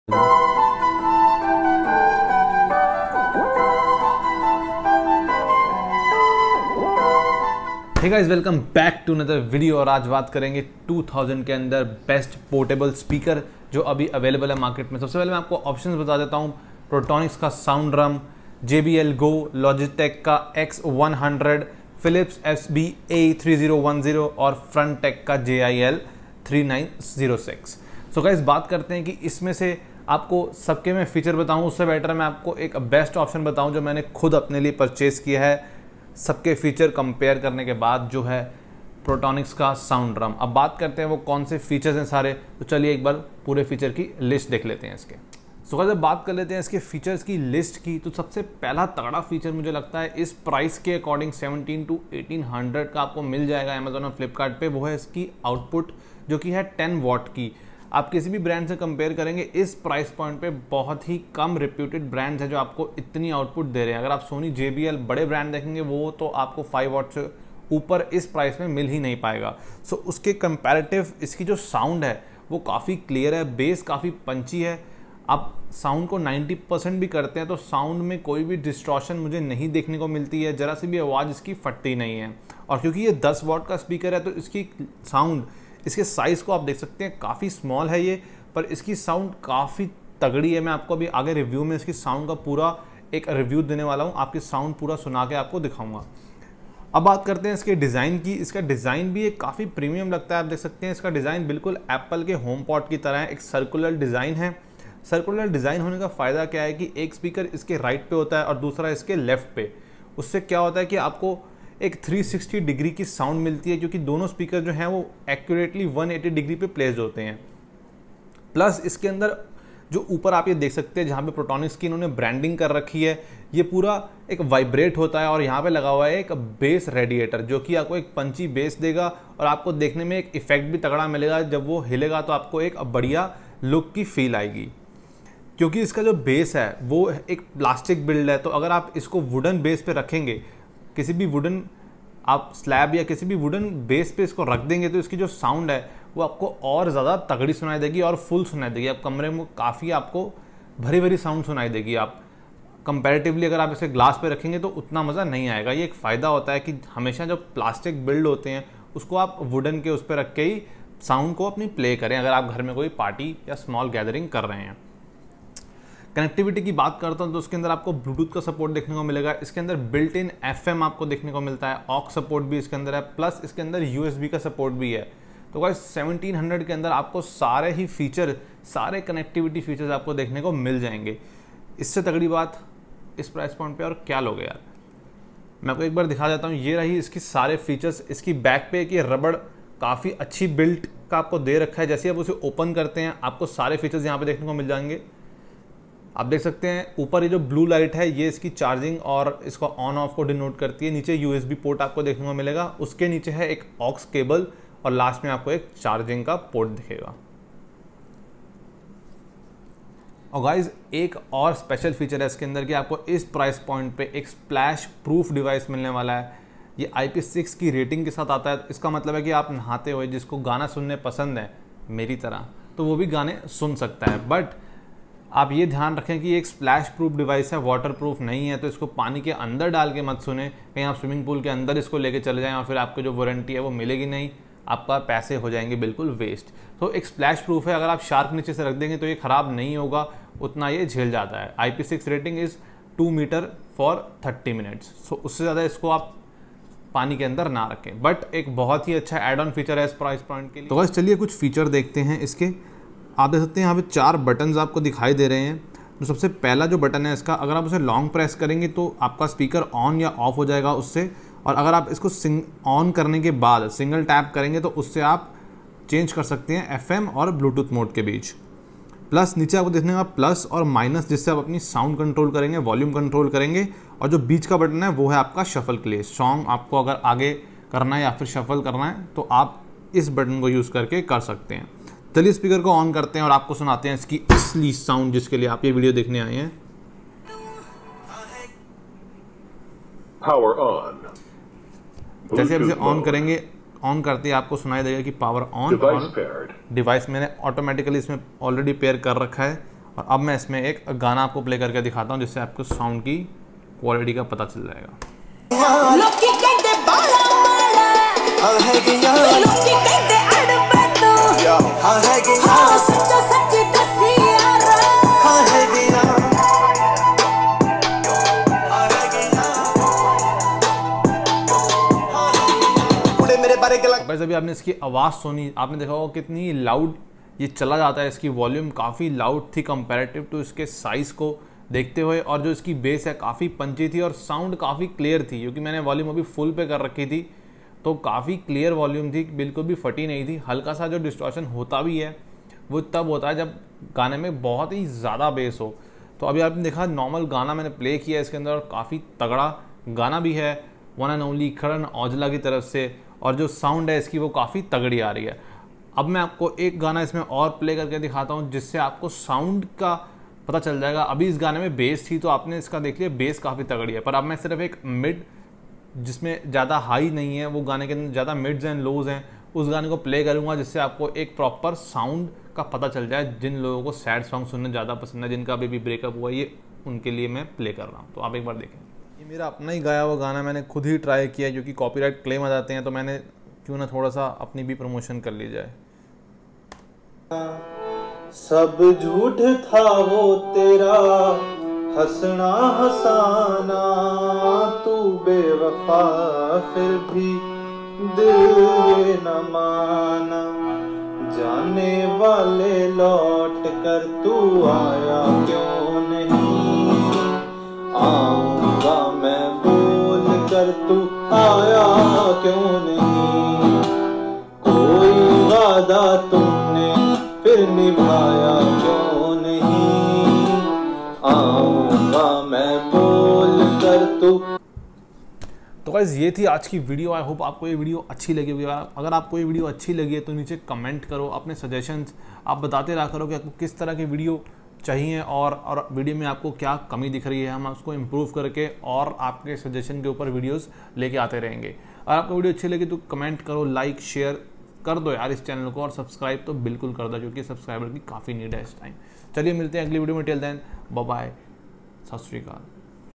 Best portable/bluetooth speaker under 2000 in July 2020. (Hindi)
He guys in this video I have shown Top Bluetooth Speakers Under Rs 2000.
In this video, you will see Best Bluetooth Speaker review among them.

Porotonics Sound drum review:-
Portronics Sound Drum Bluetooth Speaker Sound Test, Review and Bass test in Hindi. In this detailed review of Portronics Sound Drum, we are testing its sound quality (bass and vocals) and are discussing its features with pros and cons. Portronics Sound Drum is a great choice if you're looking for a powerful 10W Bluetooth speaker that is compact, offers good sound quality & battery life, has FM & Pen Drive options and is also IPX6 water resistant.